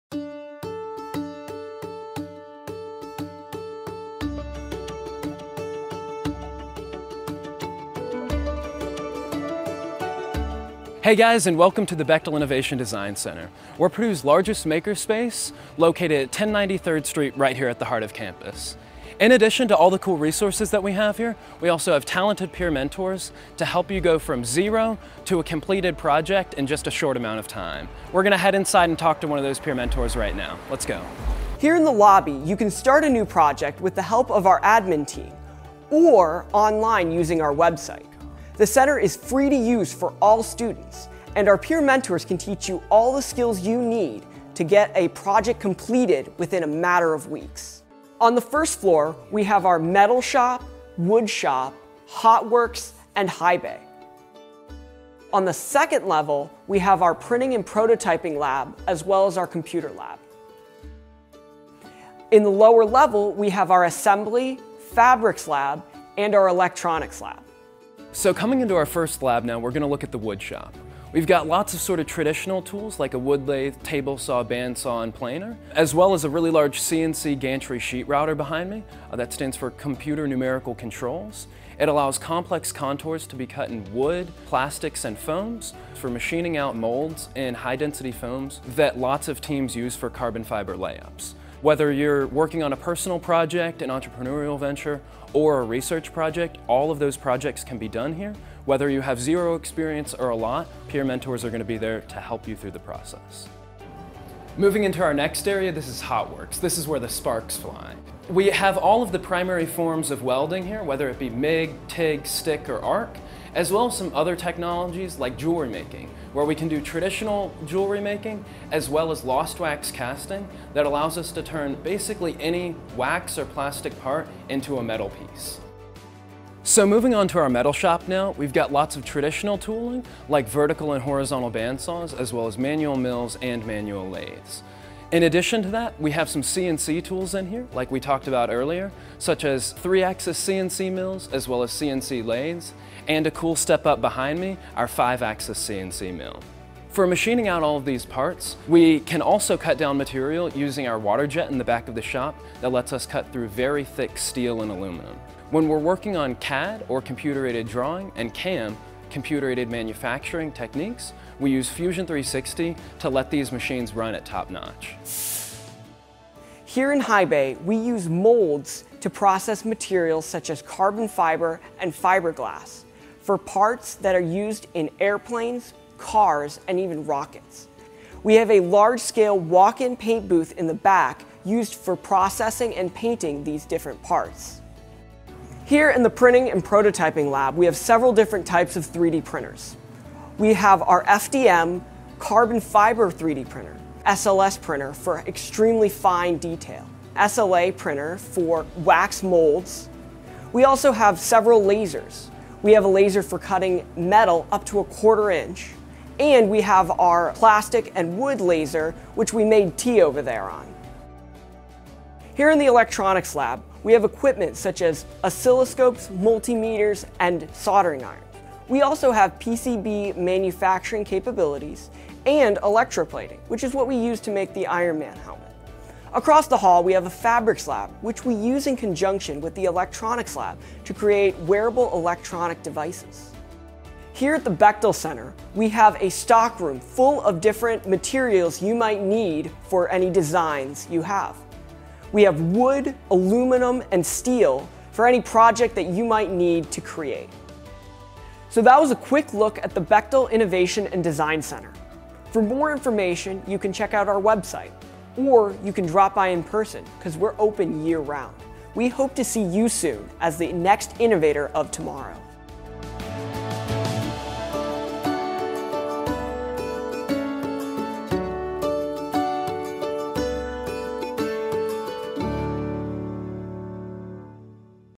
Bechtel Innovation and Design Center Facility Tour
The Bechtel Innovation and Design Center is a hub for innovation across various industries. From processing carbon fiber and fiberglass for aerospace, automotive, and aerospace parts to an array of 3D printers and laser equipment for intricate work, this facility offers a diverse range of tools. With labs dedicated to electronics, PCB manufacturing, and wearable tech, alongside a stockroom brimming with materials like wood, aluminum, and steel, the center supports ambitious projects. See you soon!

The center supports students from across Purdue as they pursue personal, entrepreneurial, societal, team, and class related projects.